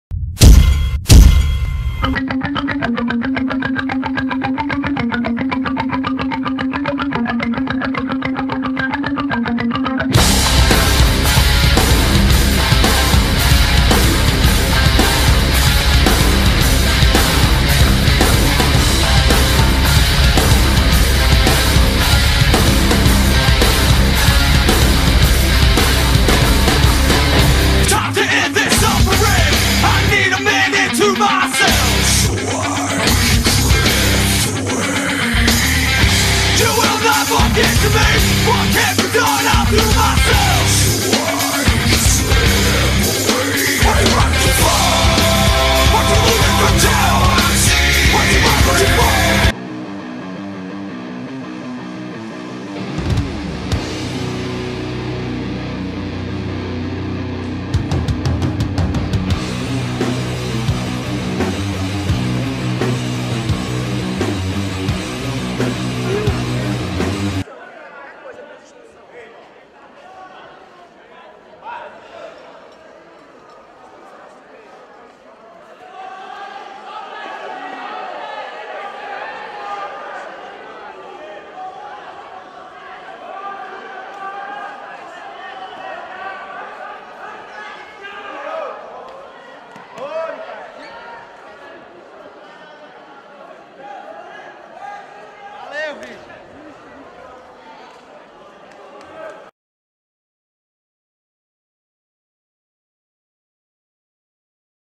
TV Meiaguarda - Campeonato Aberto de Beberibe - Abs Azul Juv - Mikael Rhailander x Ricardo Varagnat
Quer saber mais sobre Jiu-Jitsu no Ceará e no Nordeste? Acesse